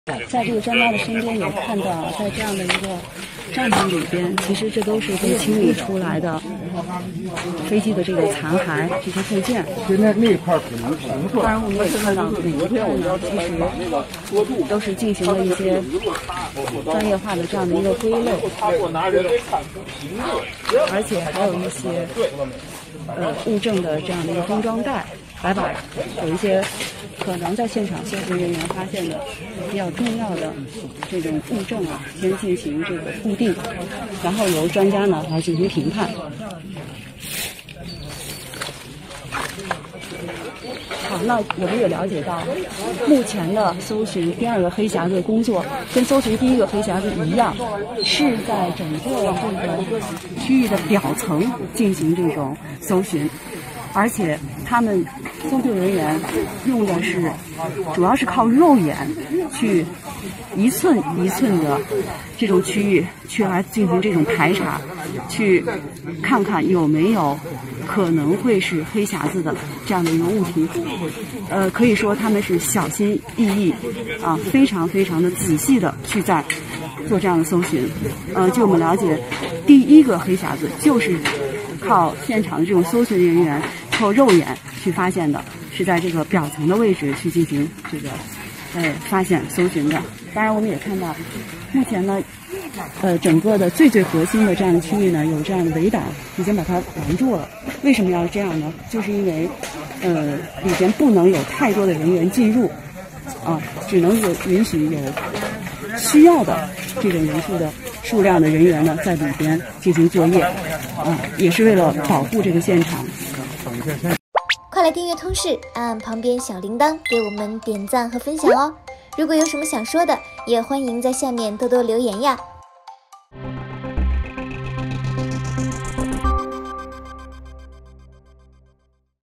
東航失事飛機的黑匣子原來是在雨中靠肉眼找到的！
據央視記者現場報道，3月24日，MU5735航班的搜救人員已將不少搜尋到的殘骸分類，以作為後續調查評判的物證，當中最重要的飛行記錄器“黑匣子”目前已找到一個，現場人員仍在搜尋第二個黑匣子。東航空難